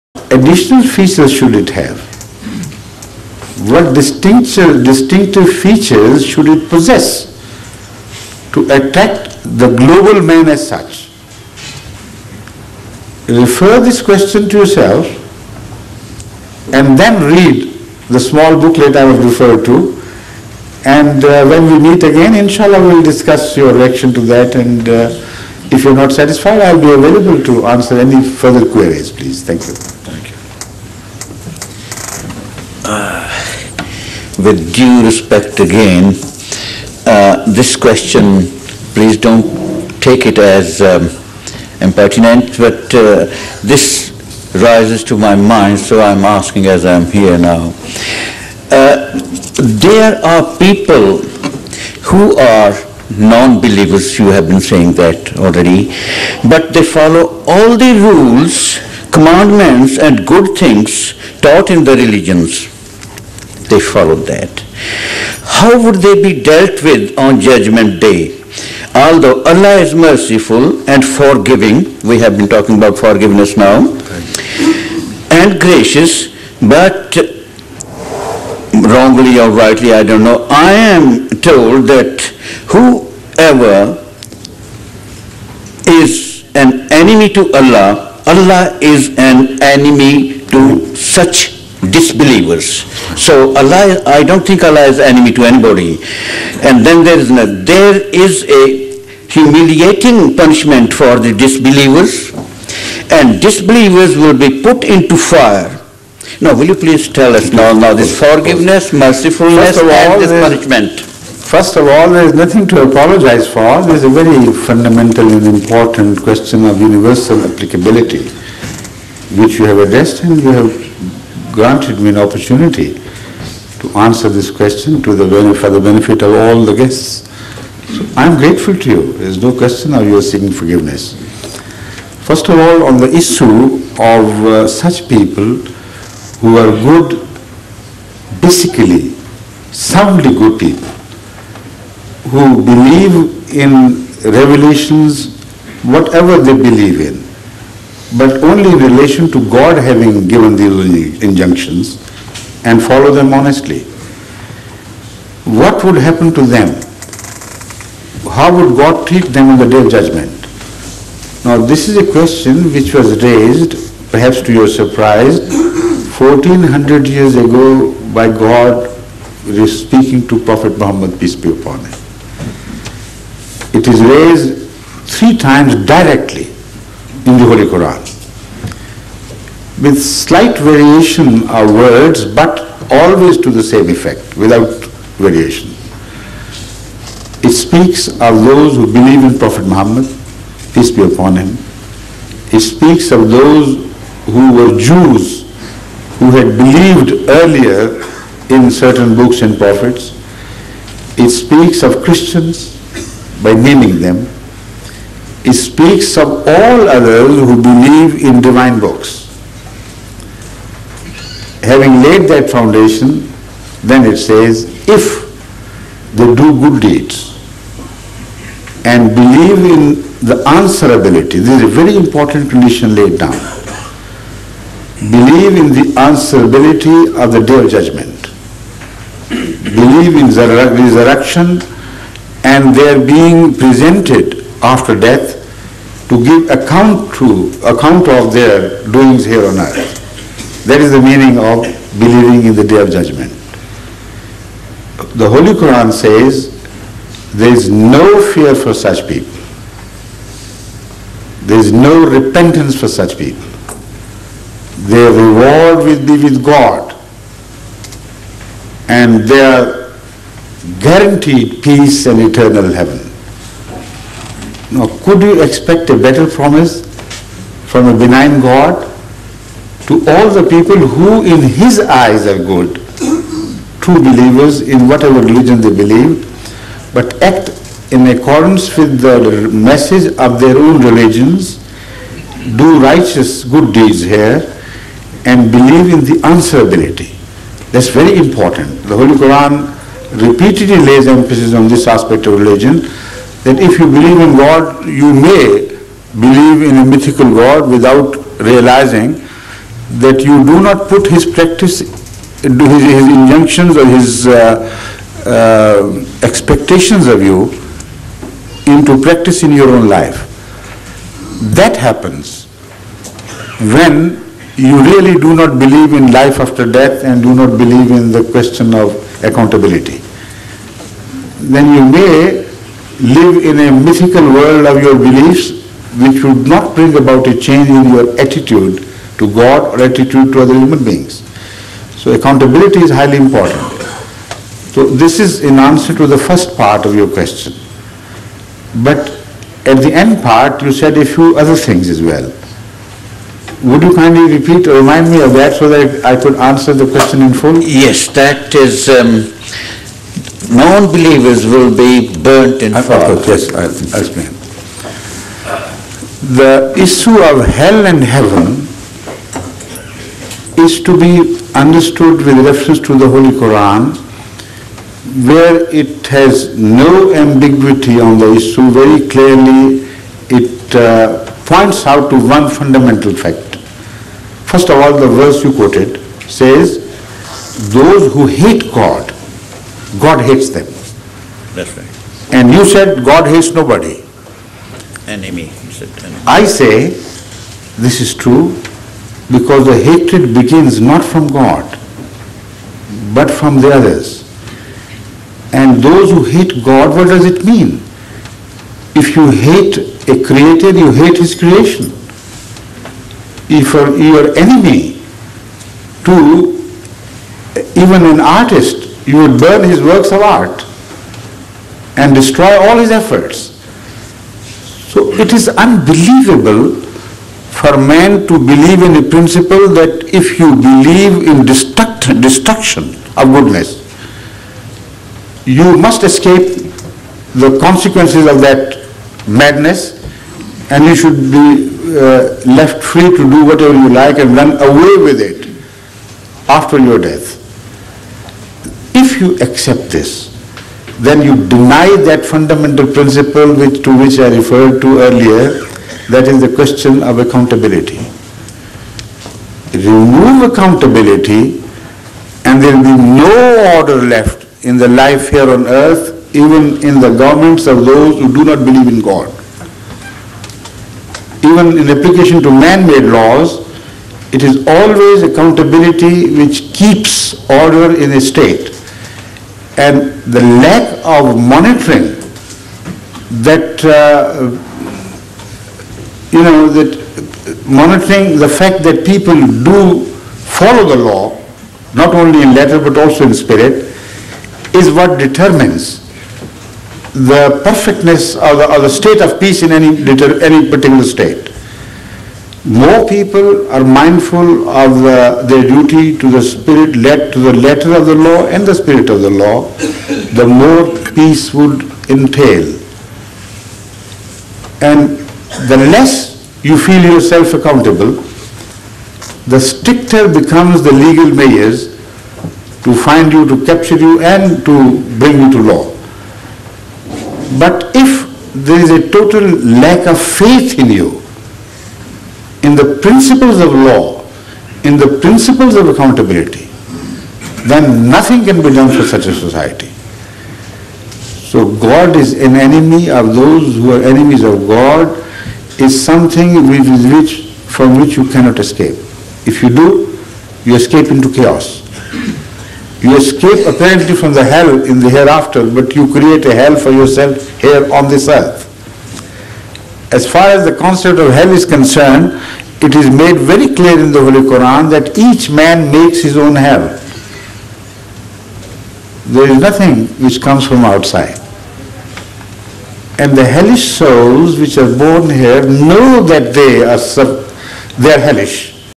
Islam - English Q/A session - 1995-01-29 - Part 7 of 15
Ahmadiyyat

Questions put to Khalifatul Masih 4th, Hadhrat Mirza Tahir Ahmad (rh)

2.
4.
5. (more)